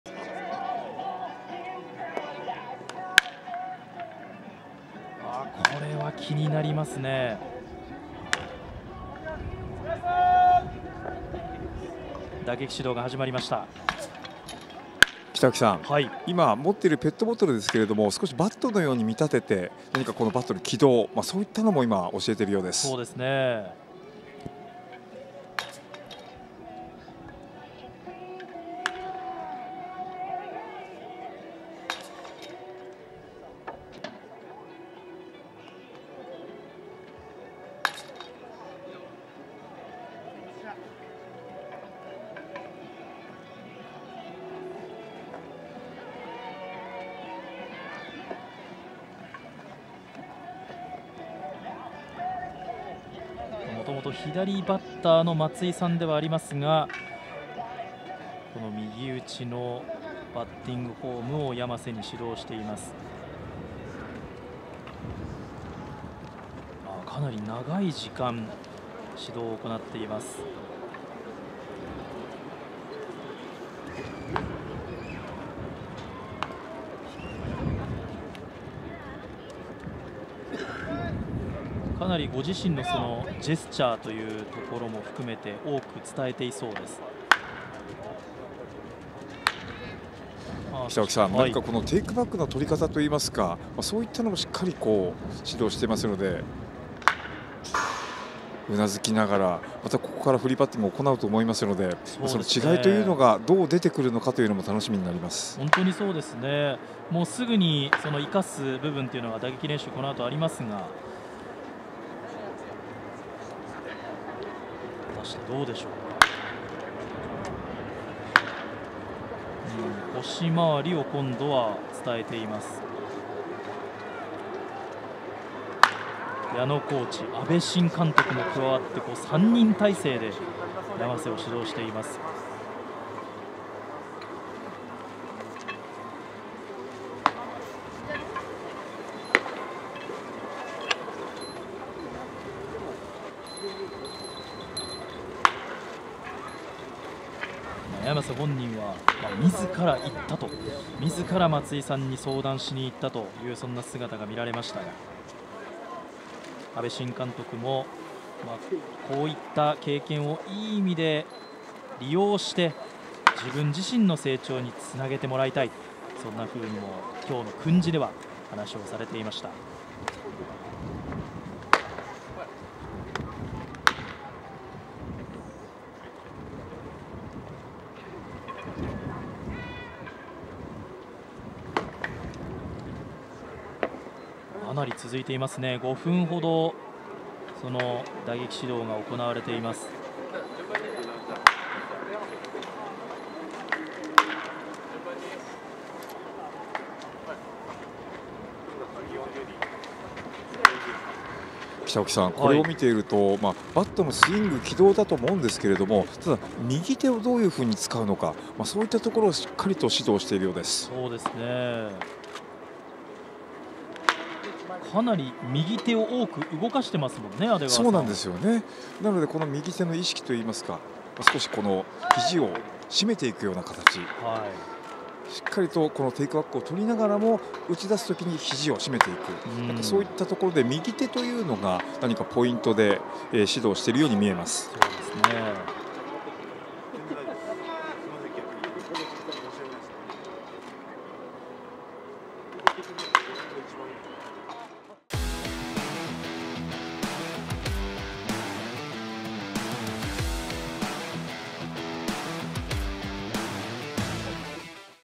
【春季キャンプ】松井秀喜臨時コーチ打撃指導"山瀬慎之助"【巨人】
2024年2月10日(土)
第3クール1日目